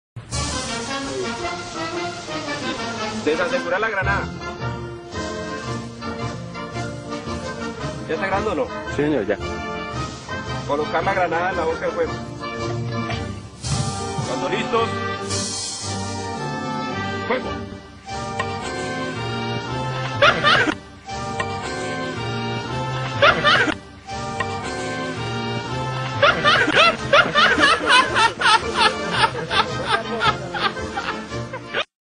Mexican army fail.
The mexican army failing with a mortar bomb.